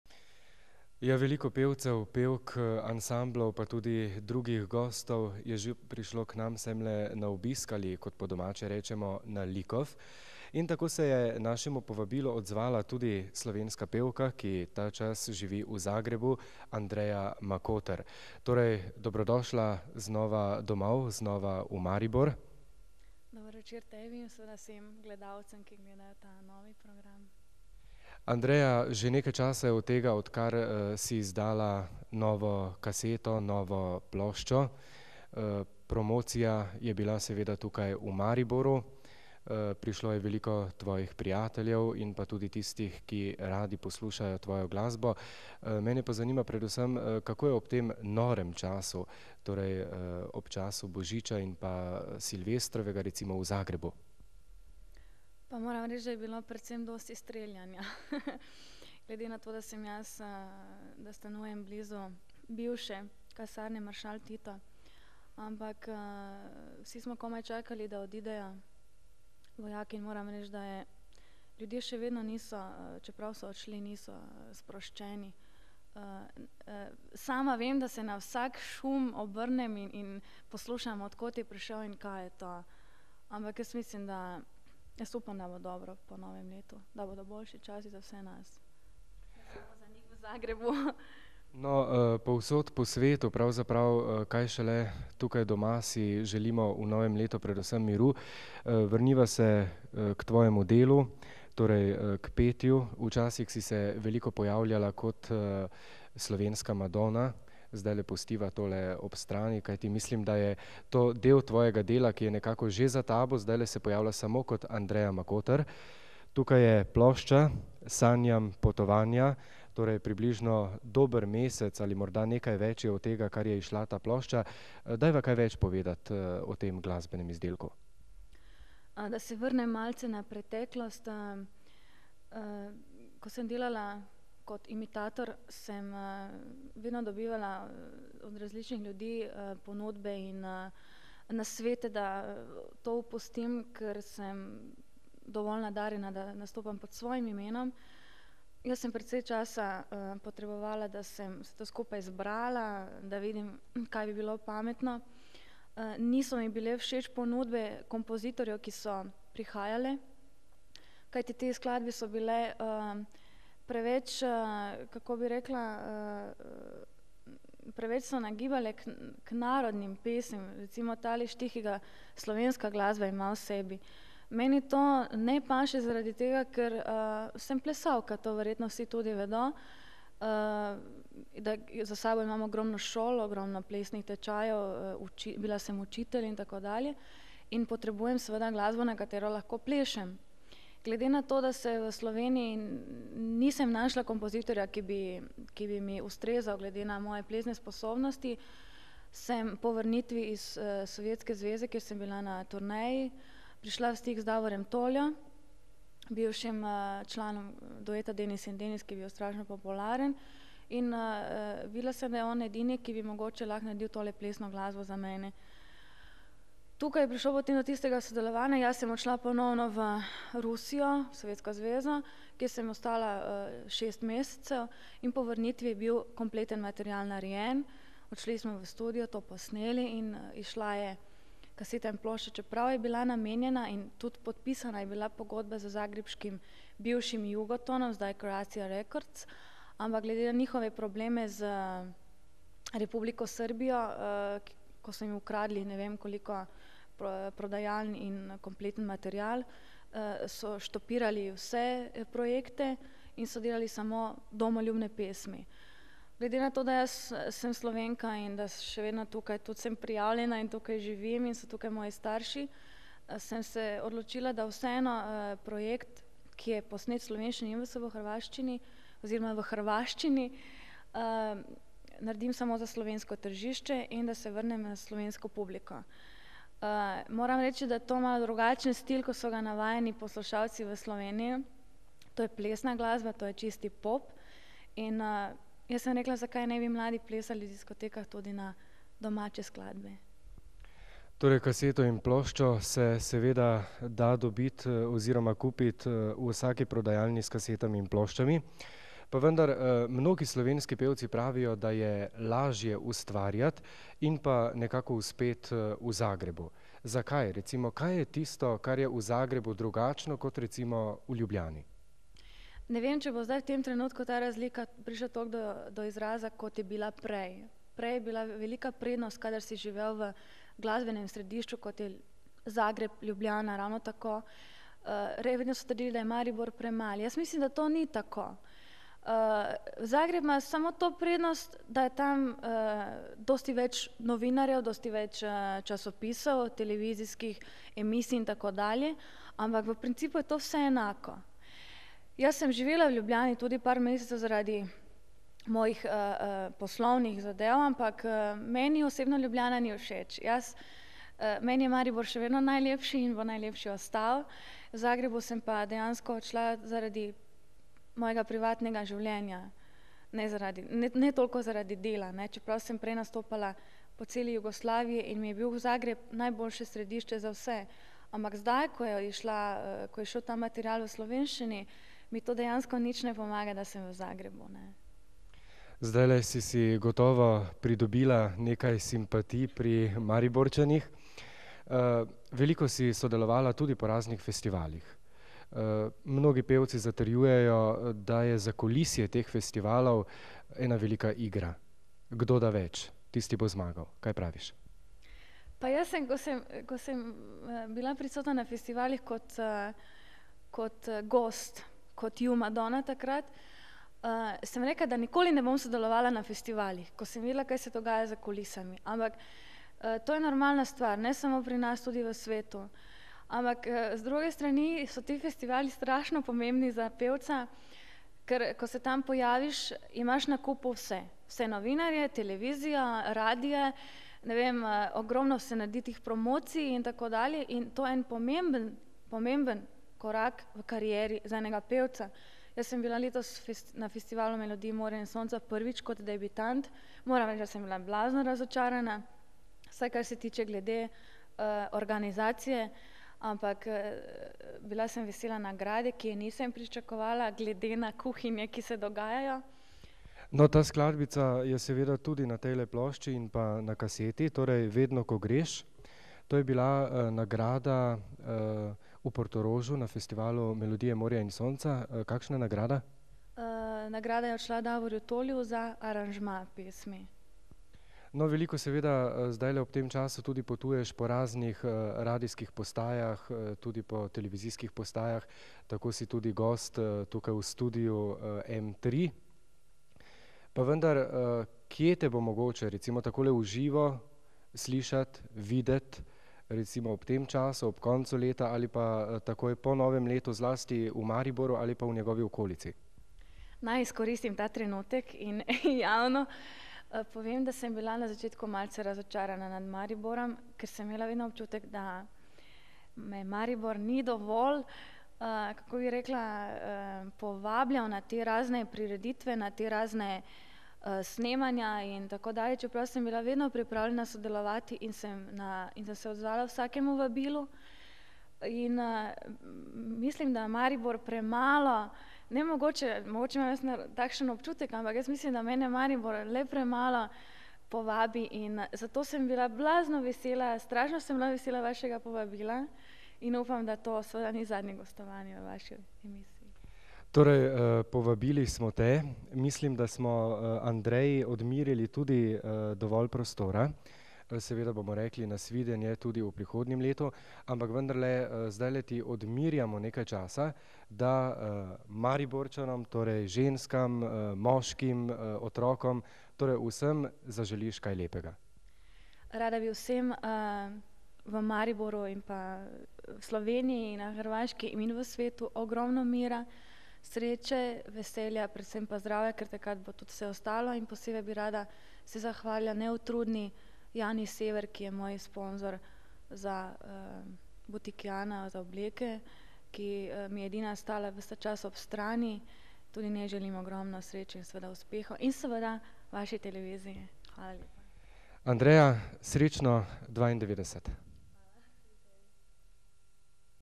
Andreja Makoter - pogovor v studiu M3, dec. 1991
Takrat je postajala zvezda za celo Jugo in ko se je vračala v Maribor tudi povedala nekaj o sebi in glasbeni poti... Njen instant spot pred pogovorom na